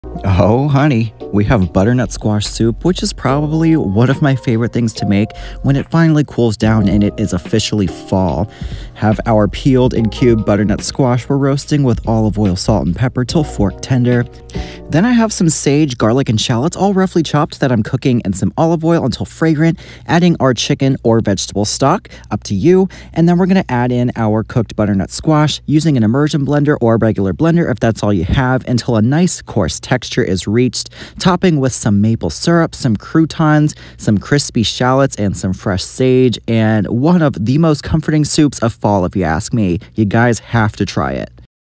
Butternut Squash Soup
I love Fall for probably a million reasons. One of them being that I can make tons of comforting soup to pair with the cooler weather. This butternut squash soup is probably one of my top 3 favorites to make, because it’s simple, but oh honey it’s so good!

What you need:
 -2 medium butternut squash (5.5lbs), peeled, and cubed, seeds removed.
-1/4 cup olive oil
- 1 tbs kosher salt
-2 tsp black pepper
-2 large shallots, roughly sliced
-4 cloves garlic, chopped
-1/4 cup chopped sage
-1Qt chicken or vegetable stock
Optional croutons and maple syrup for topping.
Directions:
1. Preheat your oven to 375 F and line a large sheet pan with foil.
2. Put the chopped butternut squash in a flat layer on the pan, and toss with the olive oil, salt, and pepper until nicely coated. Bake for 35-40 minutes or until fork tender.
3. In a large sauce pot, heat 1 tbs olive oil and add one of the sliced shallots, the sage, and garlic.
4. Cook, stirring occasionally, for 2 minutes or until fragrant. Add the stock and bring to a simmer, then lower the heat and simmer until the squash is done baking.
5. If you want to make crispy shallots for the topping, place the other sliced shallot either in an air fryer at 400 for 8-10 minutes, or on a sheet lined with foil and bake at 425 for 8-10 minutes, until darkened.
6. Add the baked squash to the pot of simmering stock, stir to combine, then you can either pour it into a blender or use an immersion blender to blend the soup together until is has a nice coarse texture. Taste for salt.
7. Ladle the soup into bowls, top with a small drizzle of maple syrup, some croutons, and crispy shallots!

This recipe is enough for about 4 people but can easily be multiplied or cut in half!

And don’t forget to follow me and like/share my recipes! Thanks!